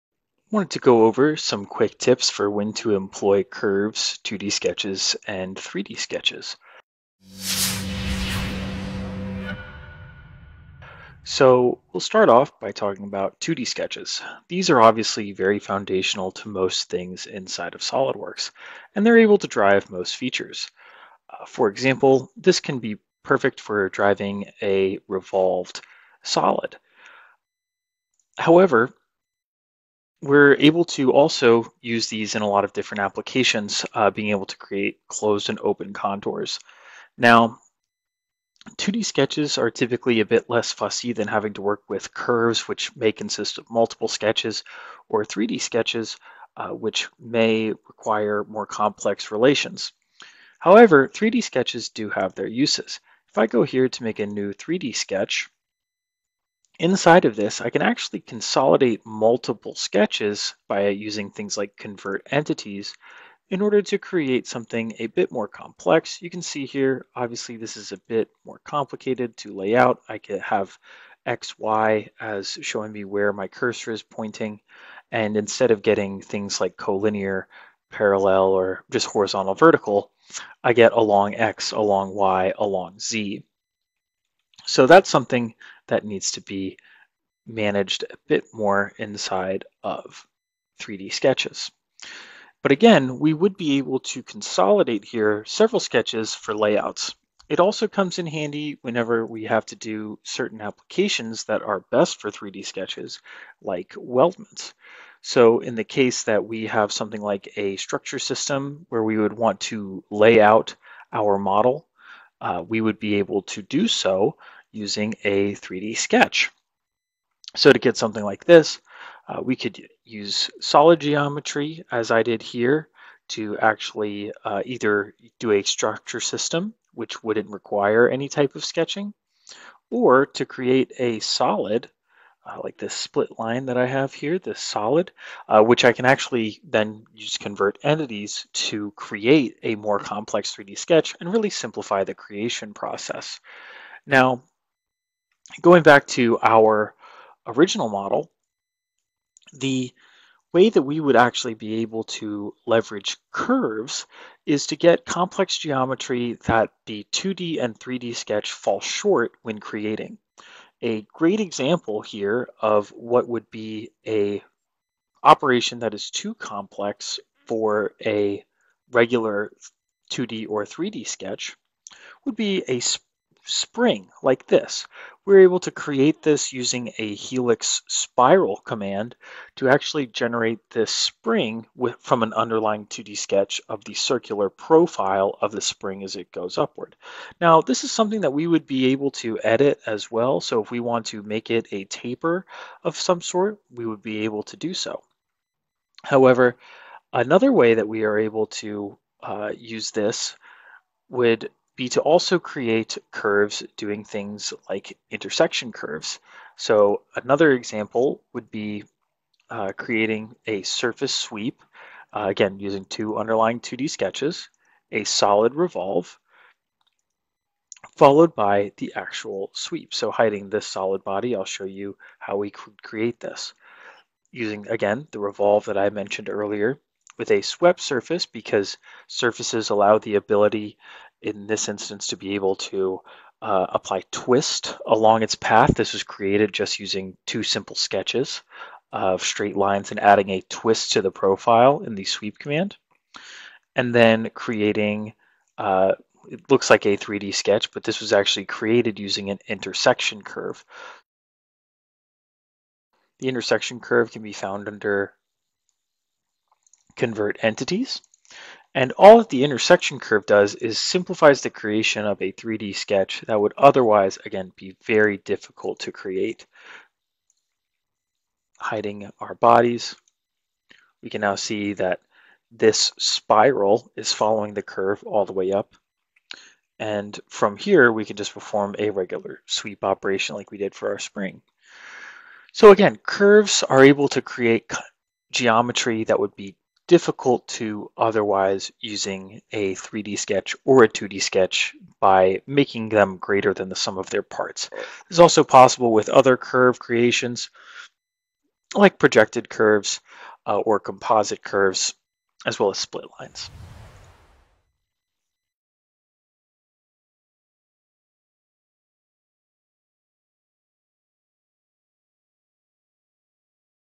SOLIDWORKS Tech Tip: When to Employ Curves to 2D and 3D Sketches
This video reveals a tech tip that will enable to create cool stuff way faster. Watch this video to see when you should use curves and when you shoulnt.